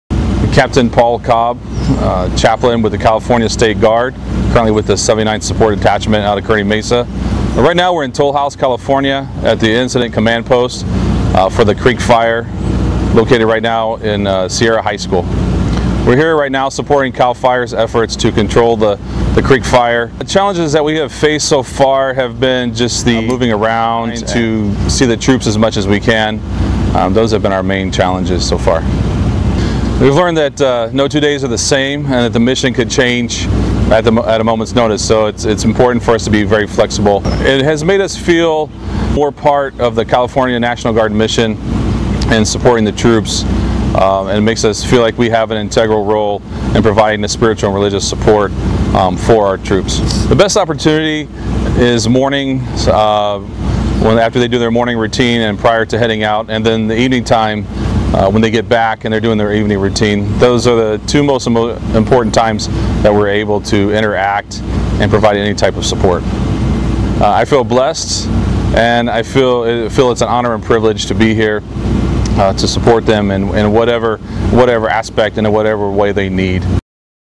Chaplains assist service members in the field
Chaplains assist service members in the field supporting wildland firefighting operations throughout California. (U.S. Army National Guard video by Sgt. 1st Class Ryan Sheldon)